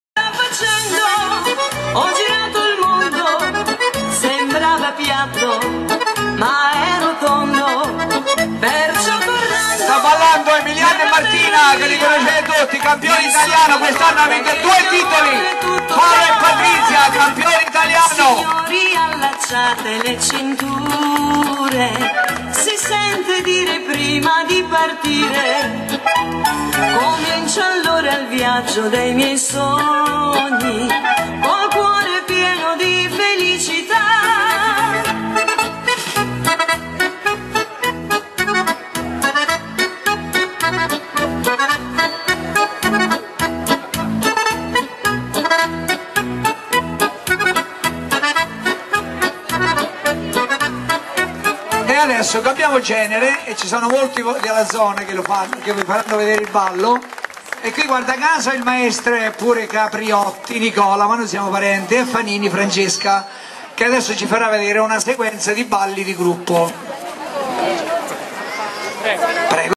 00348
Esibizione ballo-Liscio e di gruppo.Maestri Mario e Marco Capriotti.Francesca e Nicola Capriotti.Della scuola di ballo Palma D'oro di S.Benedetto del Tronto.17-07-2011-